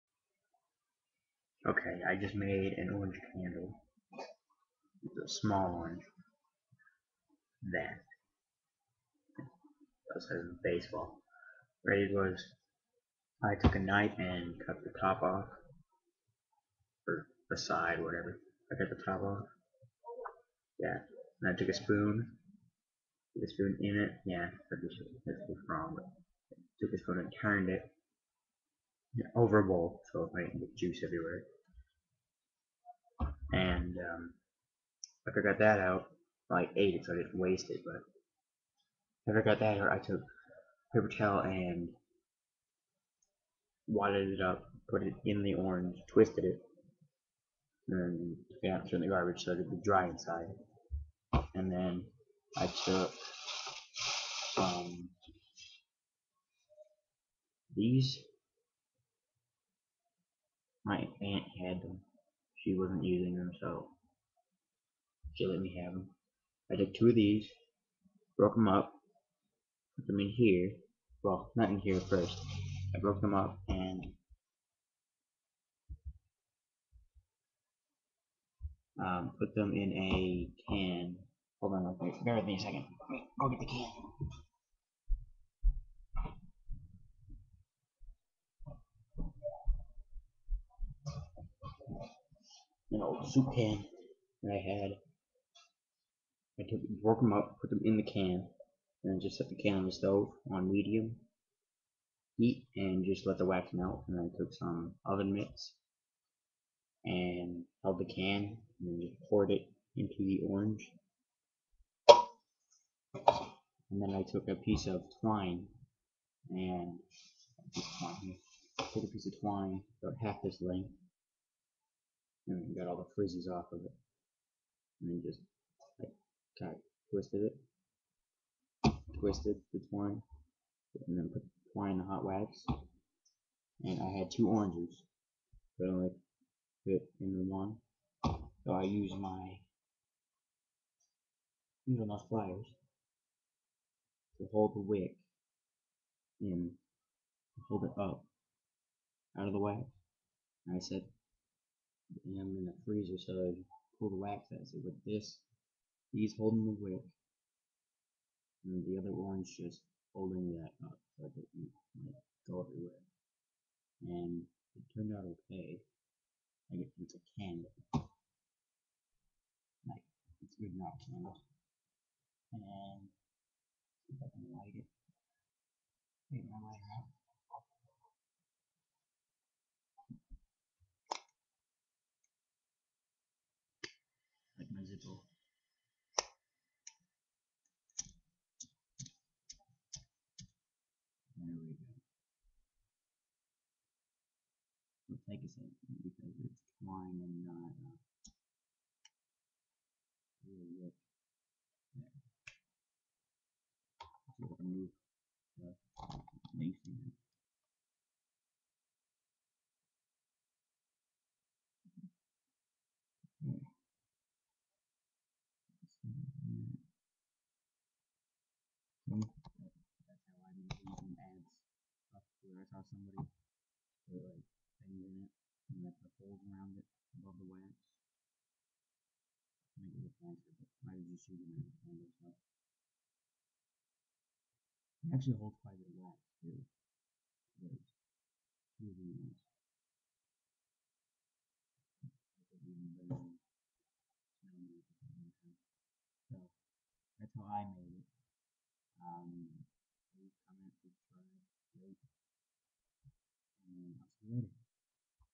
How to make an orange into a candle!
This video is about how i made an orange peel into a candle. Thanks rahulvaghela999 for the idea!